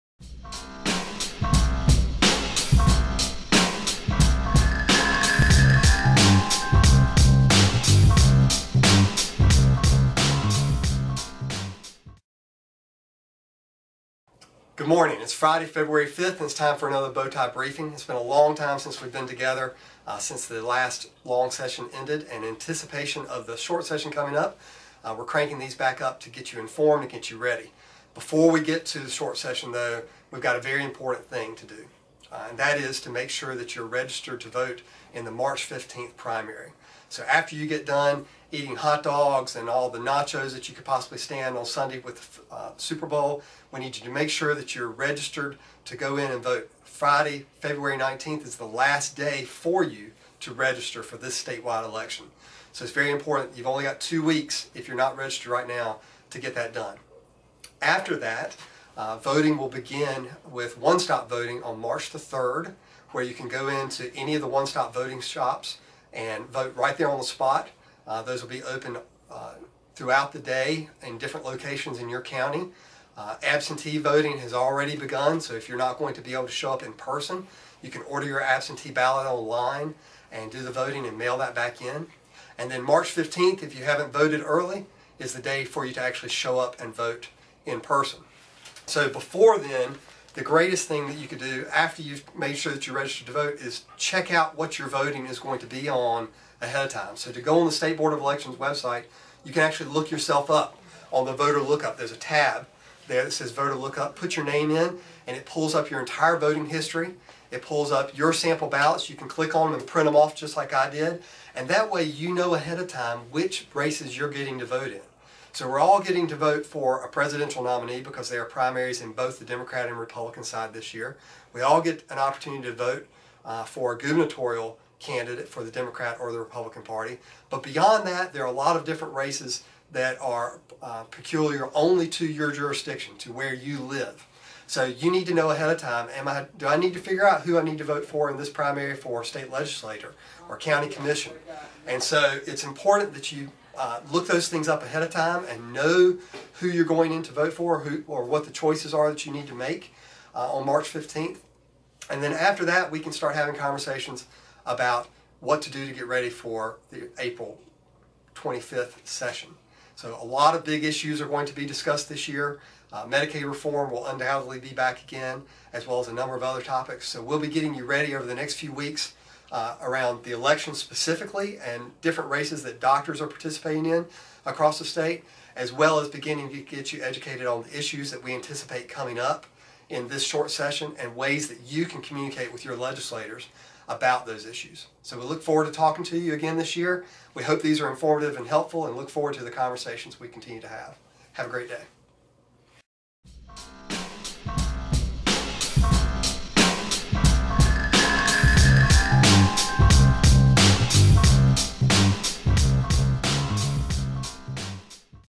Chip Baggett's Bowtie Briefing for February 5, 2016
Chip's legislative advocacy series starts up for 2016 with a look at what you should do before the upcoming primary elections.